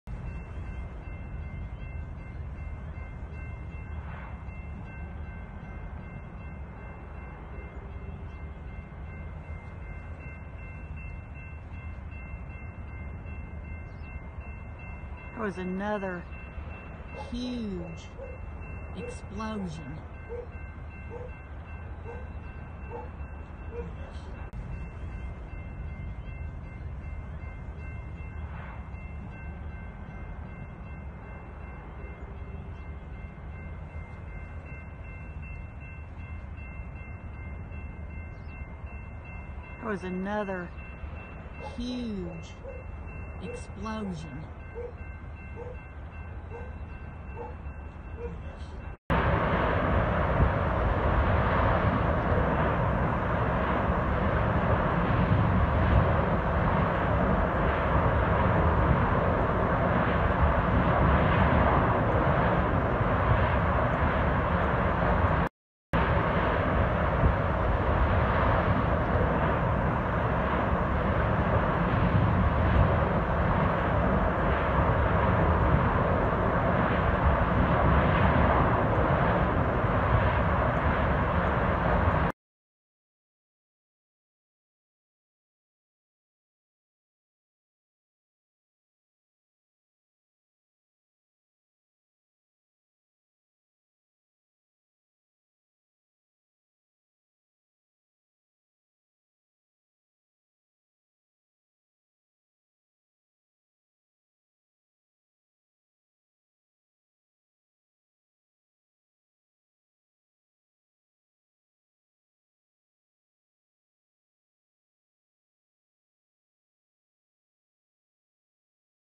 Train crash in Texas Train carrying fuel tank cars collides with 18 wheeler
Texas Train crash in Texas Train carrying fuel tank cars collides with 18 wheeler